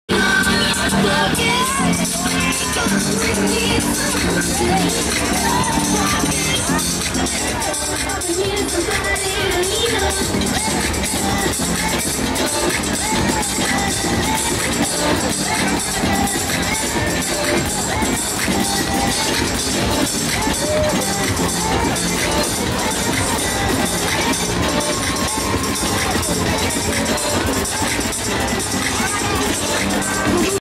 37 MARCHA DEL ORGULLO LÉSBICO GAY (MÉXICO, D.F., 2015) #33
Marcha del orgullo lésbico, gay, bisexual, transgénero, travesti e intersexual (LGBTTTI), el 27 de junio de 2015 en el Angel de la Independencia.